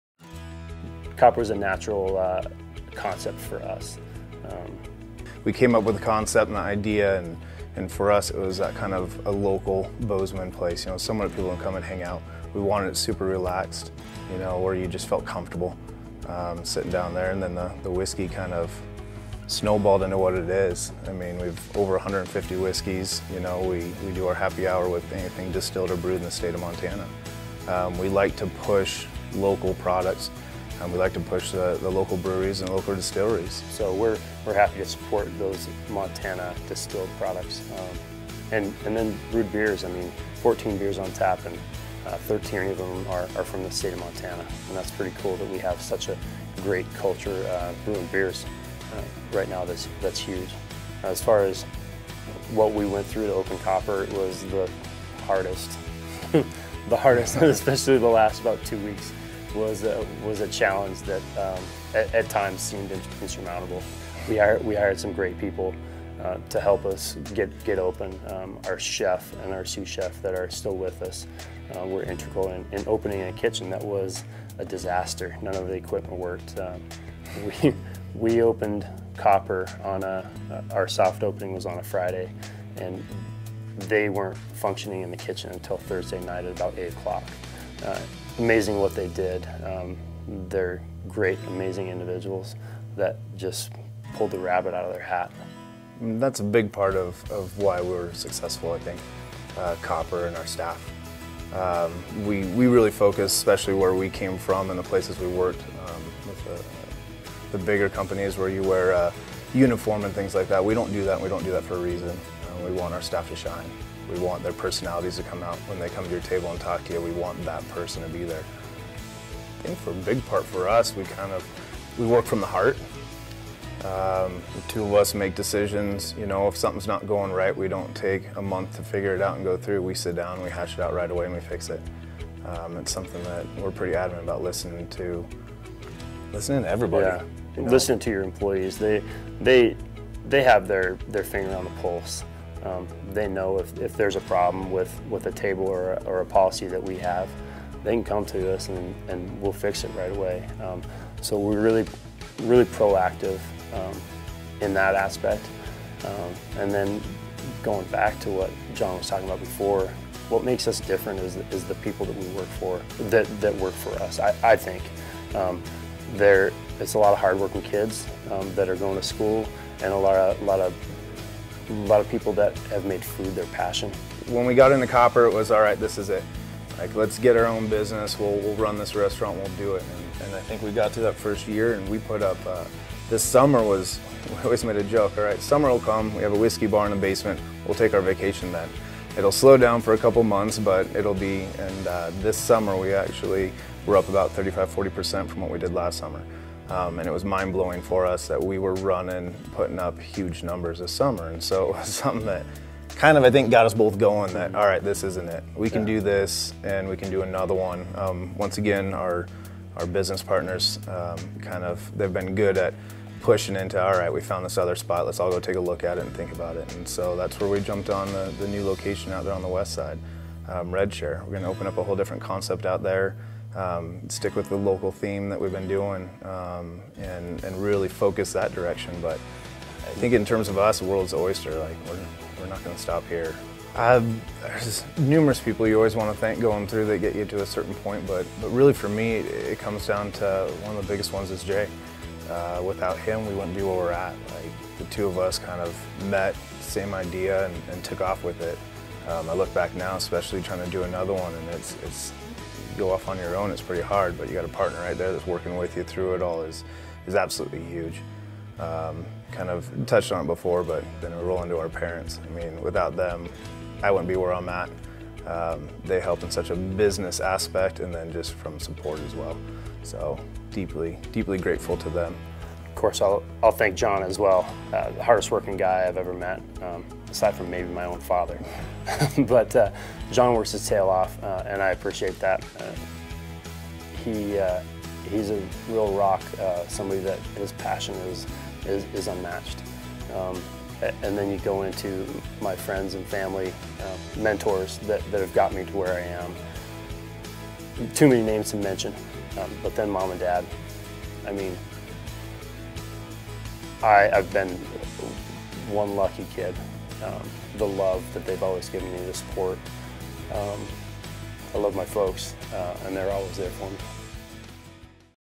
2014 Entrepreneurs of the Year: Jon Slye and Jay Thane, Copper Whiskey Bar & Grill
The Business Excellence Awards recognize businesses and individuals in five categories for their exceptional contributions to the region's business community.